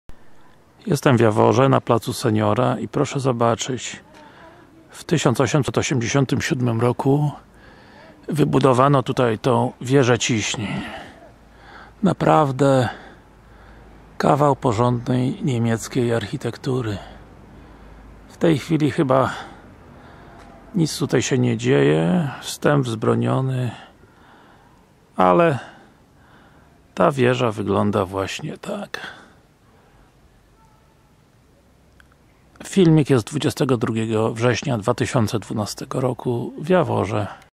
Zabytkowa Wieża Ciśnień w Jaworze
Zabytkowa wieża ciśnień w jaworze na Dolnym Śląsku. Wybudowana w 19 wieku i od tego czasu nieprzerwanie dostarcza wody miastu. Piękna poniemiecka ceglana budowla ostatnio odrestaurowana z funduszy unijnych.